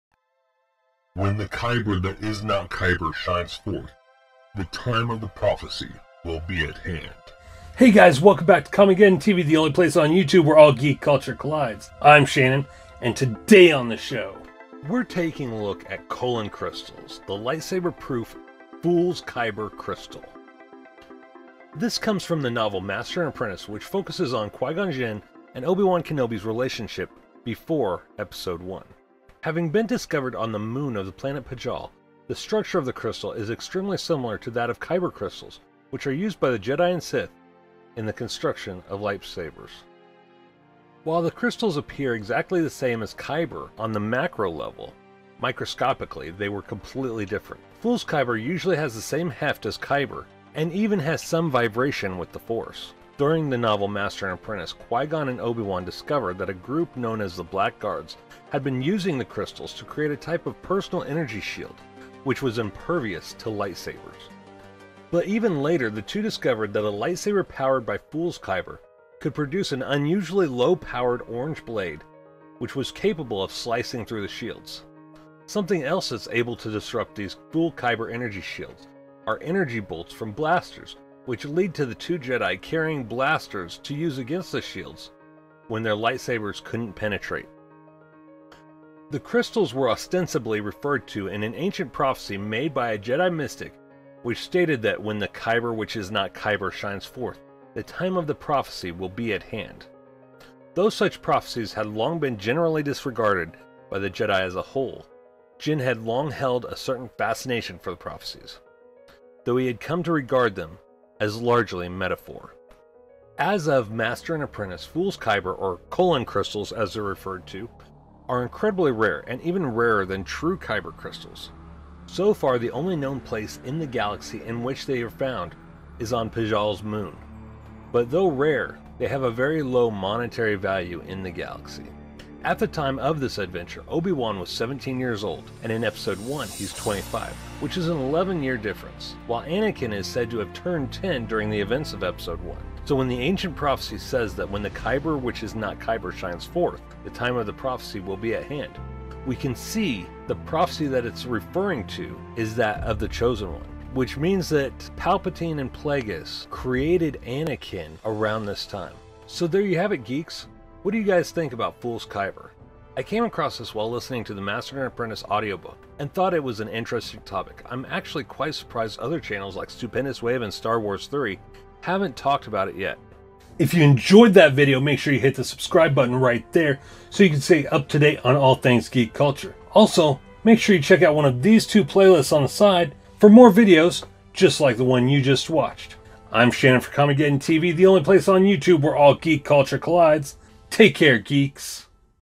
What is Fool's Kyber Kohlen Crystals | Star Wars Explained
This Video is NOT made for kids, This video is for adult collectors What is Fool's Kyber | Star Wars Explained
from the novel Master and Apprentice we find out about Fool's Kyber or Kohlen Crystals as they're named and the prophecy which accompanies them.